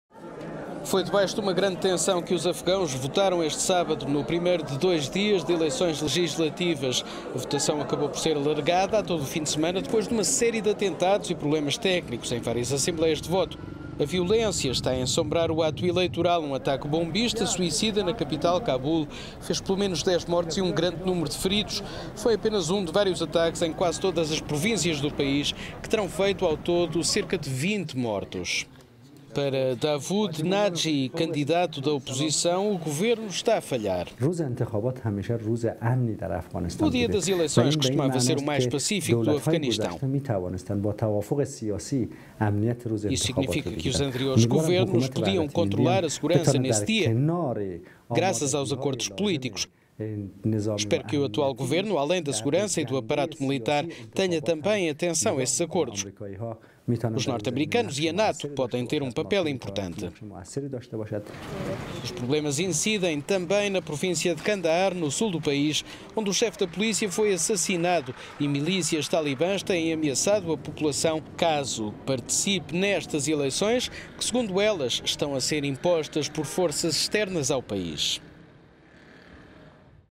Afegãos votam sob alta tensão
Os atentados marcaram o primeiro de dois dias de eleições e fizeram cerca de 20 mortos.…